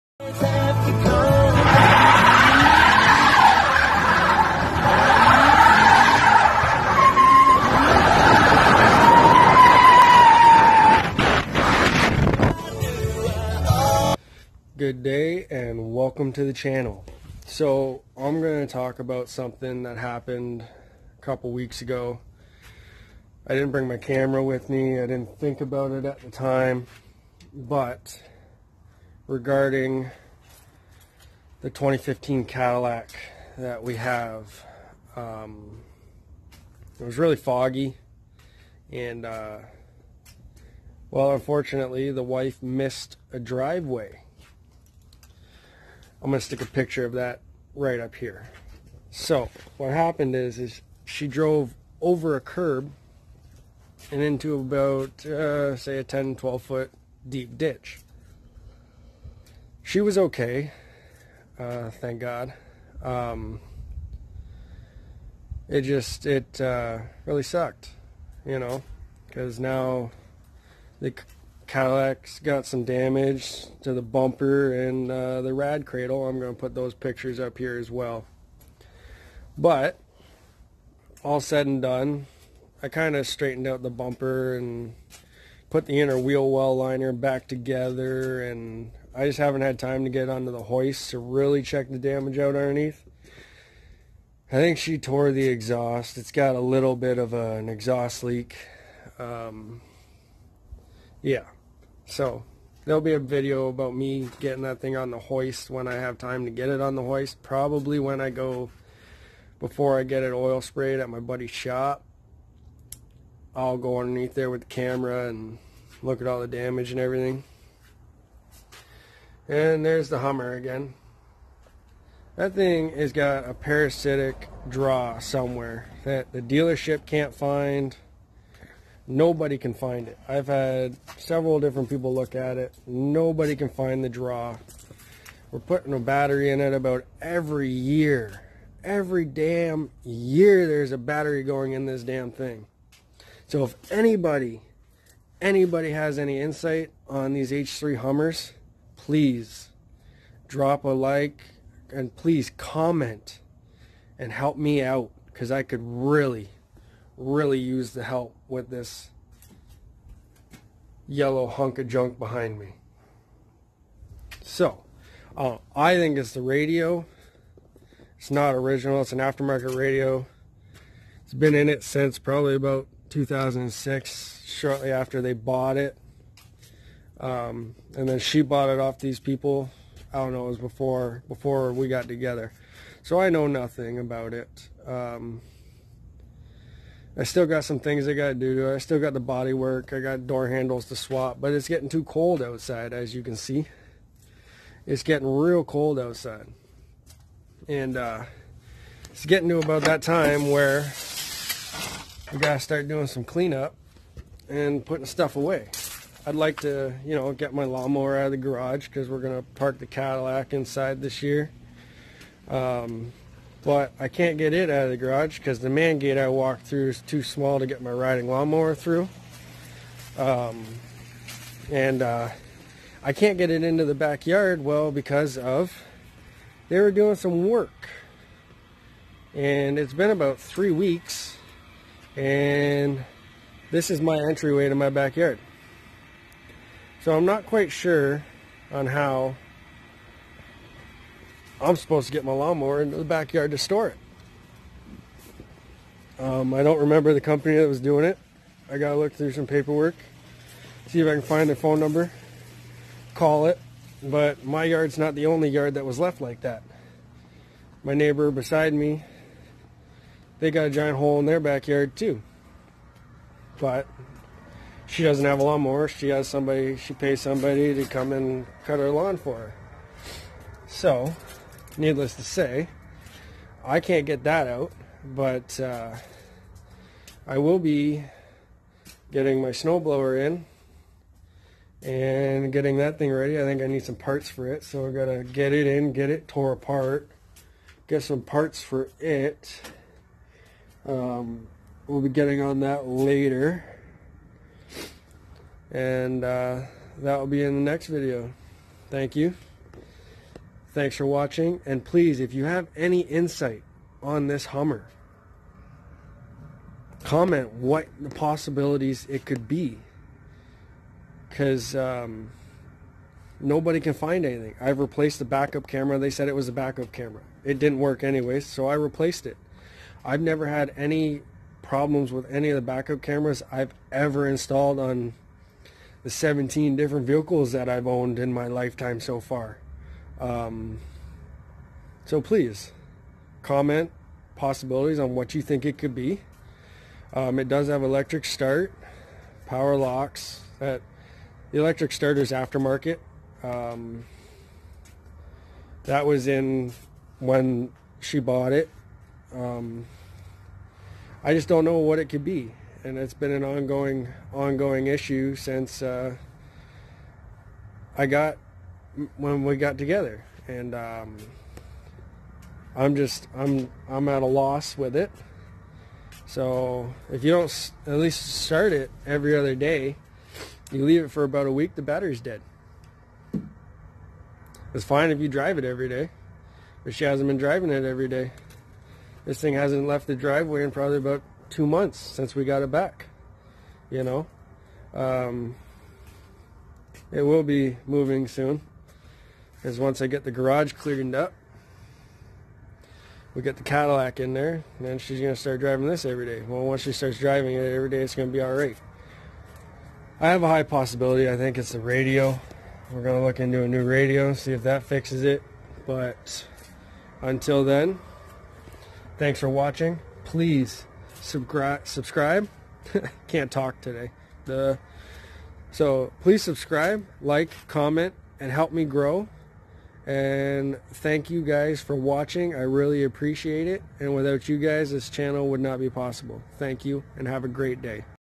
Wow ITS TOTALED. After 2 weeks of ownership
Our 2015 Cadillac is wrecked after 2 week. Everyone in the car was okay.